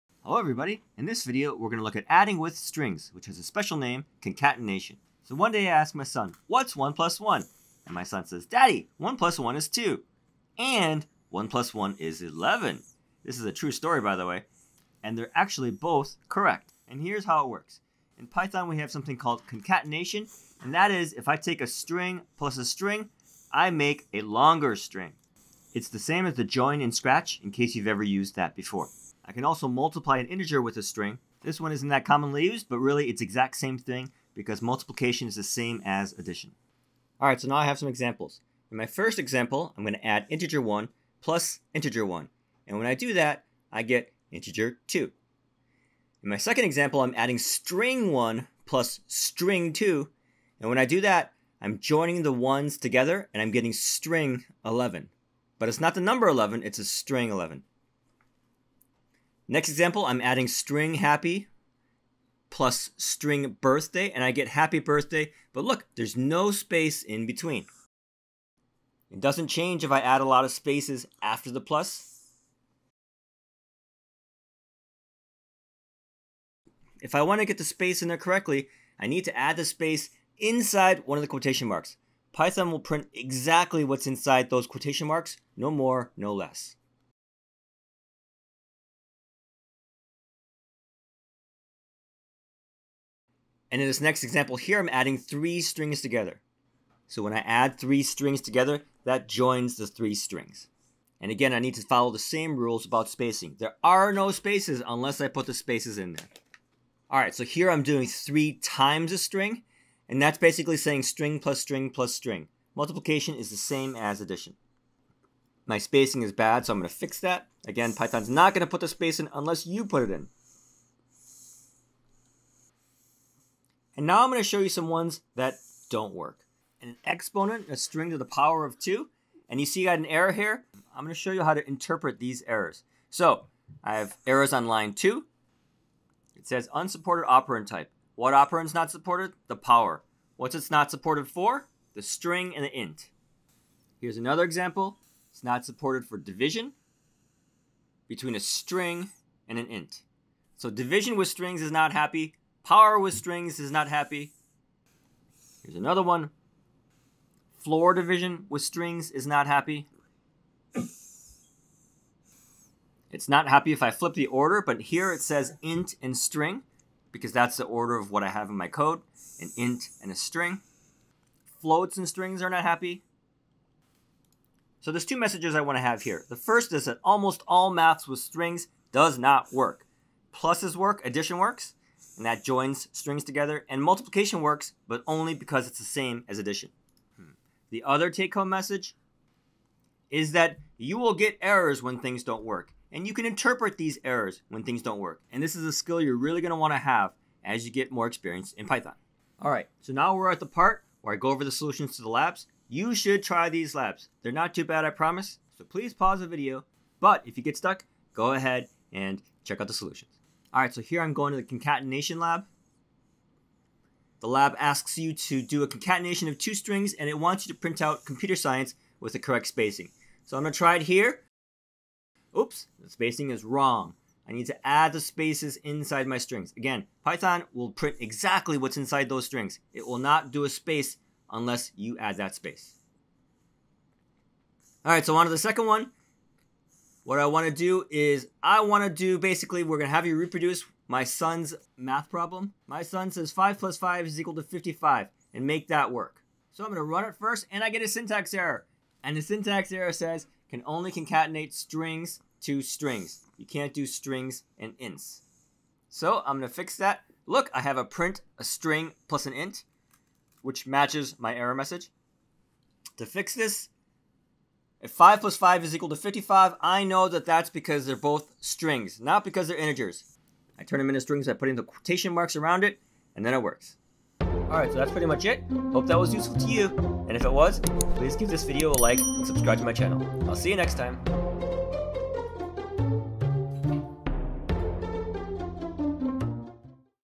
005 Concatenation in Python. Includes lab with autograding!
Learn Python with Dr. Wu!

I teach this course in my  AP Computer Science Principles class.

Learn about types in Python with two autograded labs.

00:00 Introduction
00:37 Concatenation in action
02:06 Types in actions
03:21 Solutions to the labs